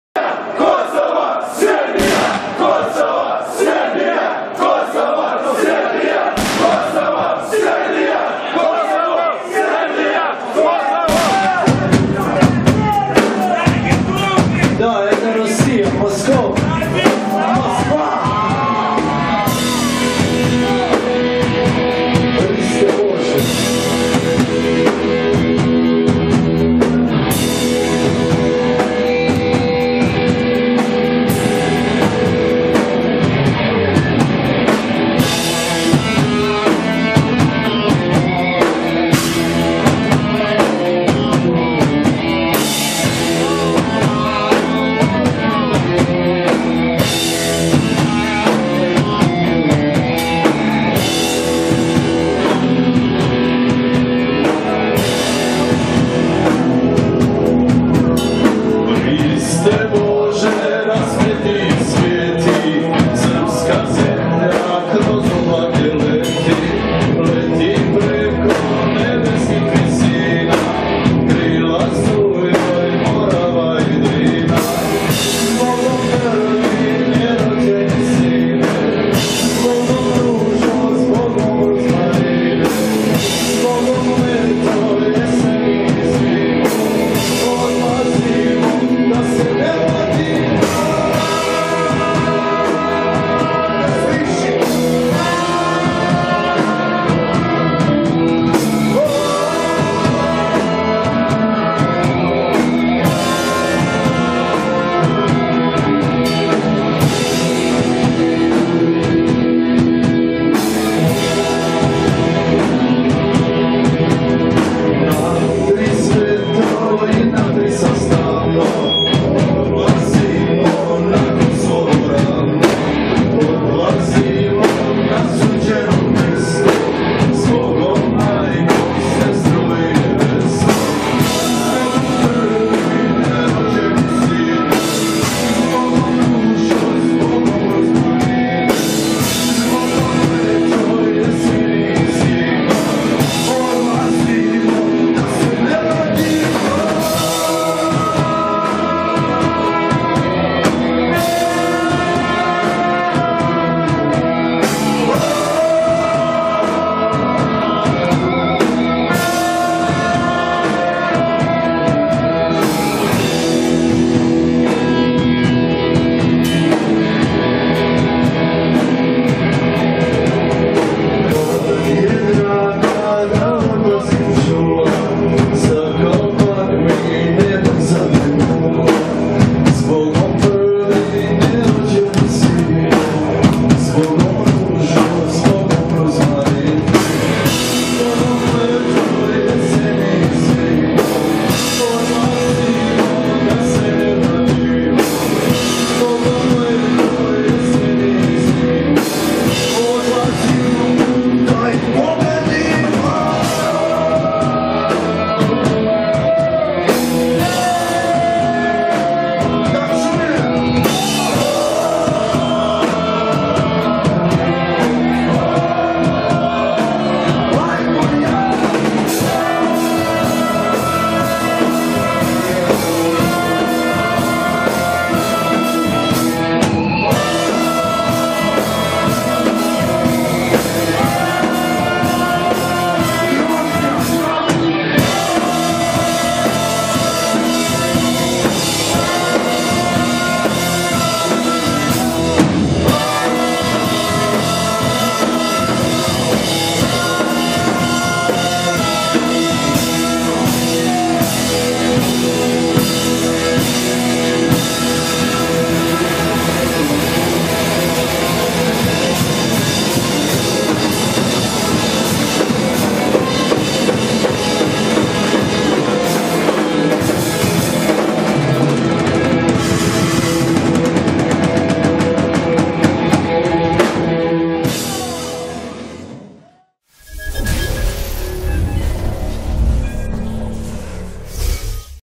Putin steht hinter Serbien
Die Zeiten von Jelzin sind vorbei. Putin steht hinter Serbien und dieses ist für den Kampf gegen die NWO bereit! Widerstand ist überall!!! Zerschlagt die NATO und ihre Vasallen!!!